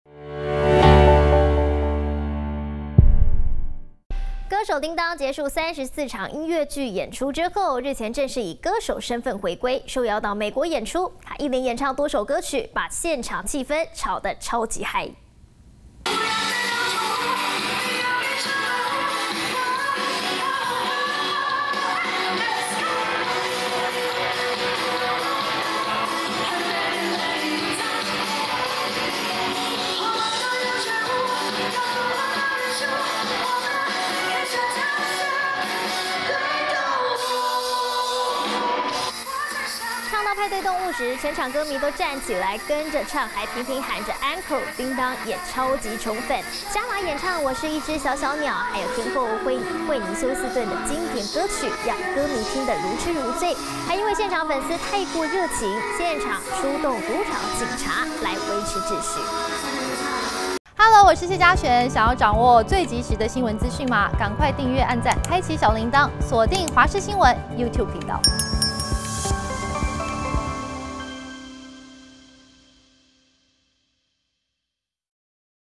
丁噹美國開唱 粉絲太熱情驚動賭城警方｜華視新聞 20230813
歌手丁噹結束34場音樂劇演出後，日前正式以歌手身份回歸，受邀到美國演出，她一連演唱多首歌曲，把現場氣氛炒得超級嗨。歌手丁噹，結束34場音樂劇演出後，日前正式以歌手身份回歸，受邀到美國演出，她一連演唱多首歌曲，把現場氣氛炒得超級嗨。